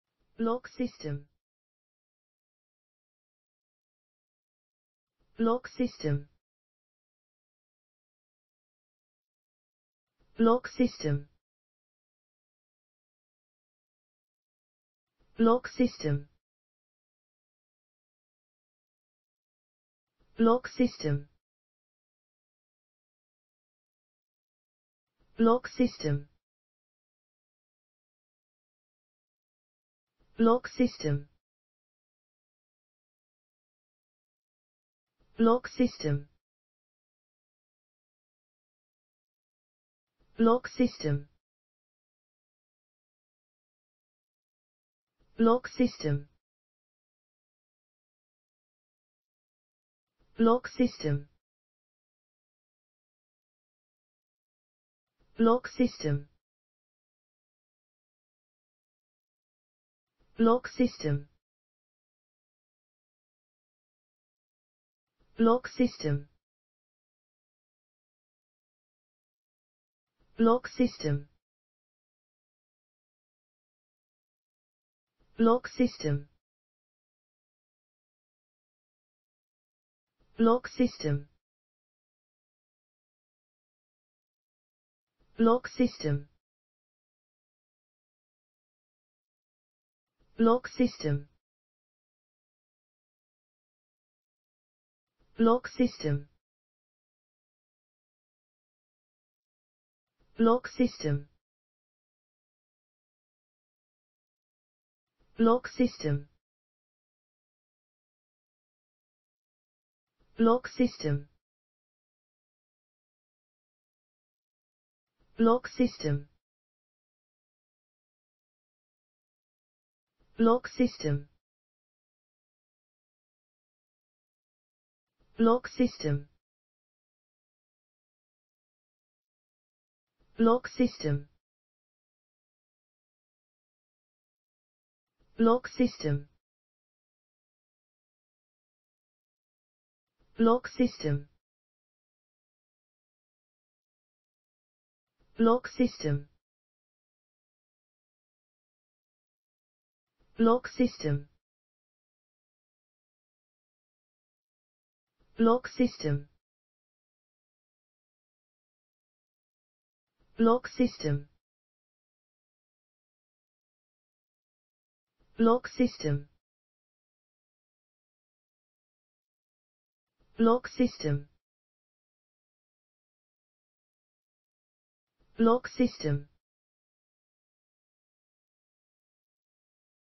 Block System. How to pronounce the english word Block System .Start with B. Learn from me.
Follow me!Learn from me how to pronounce the english word Block System.This video shows you how to pronounce the English word Block System .